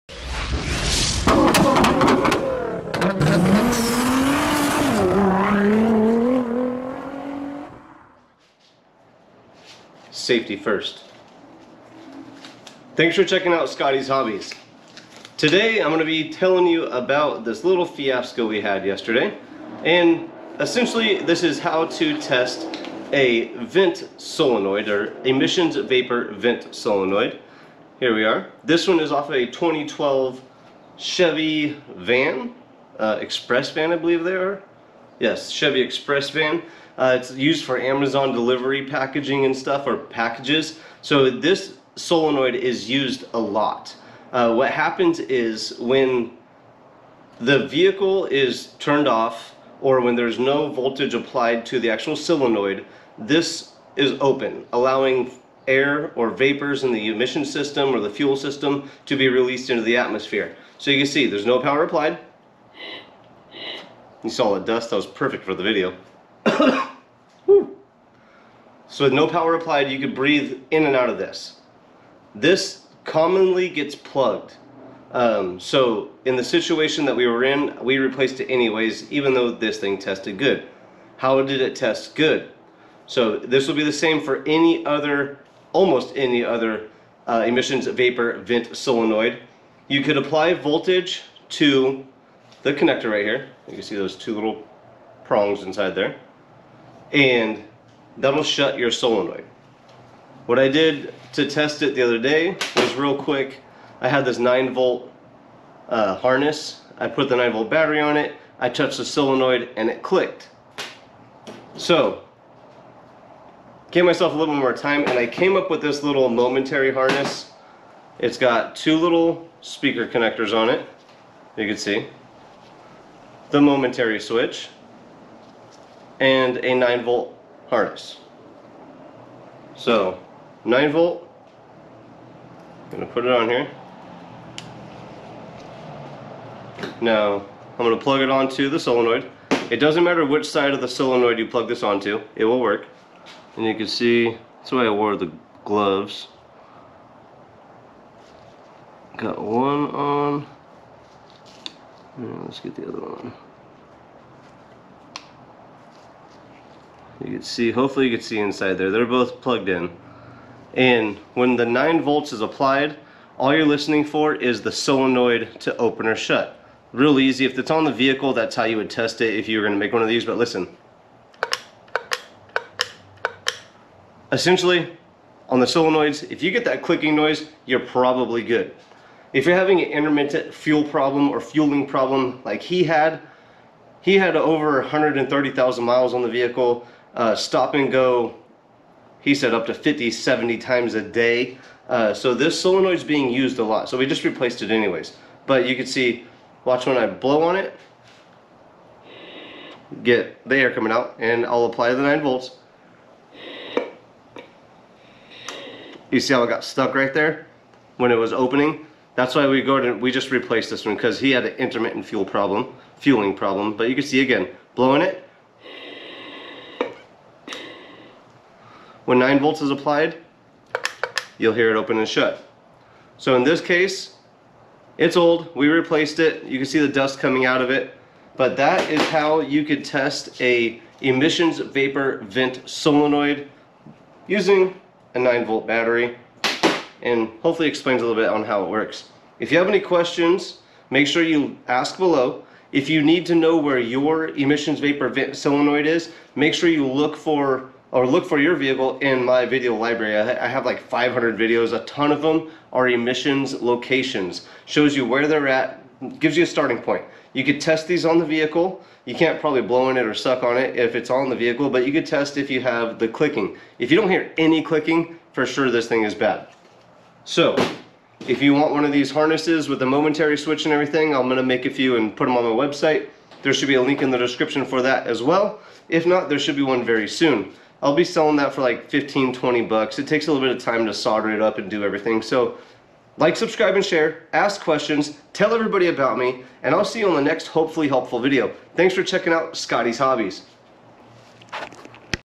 ⫷ Vapor EVAP Emissions Canister Vent Valve Solenoid. Quick info and test. p0446, p0449 ⫸
Vapor EVAP Emissions Canister Vent Valve is a common failure point of the evap system. it gets clogged and just goes bad. I'll give you some quick info how the Vapor EVAP Emissions Canister Vent Valve works and how to test it.

Facebook.com/TheRealScottiesHobbies
Check out Mitchell1 DIY, Step by step directions with torque specs
Amazon Prime free 30 day trial, Gets parts Quick!